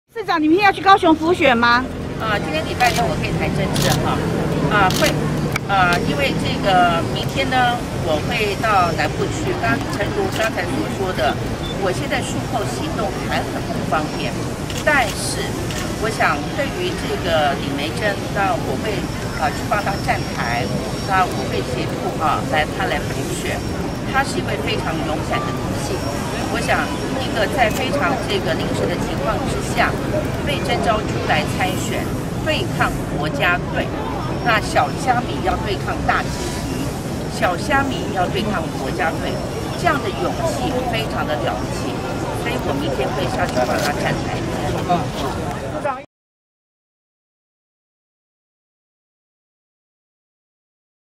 讚李眉蓁小蝦米對抗國家隊 盧秀燕：明天南下助選
高雄市長補選下週六(15日)就要投票，明天(9日)是選前黃金週，台中市長盧秀燕今天受訪時表示，會前往高雄幫國民黨候選人李眉蓁助選，因為李眉蓁非常勇敢，小蝦米對抗大鯨魚，小蝦米對抗國家隊。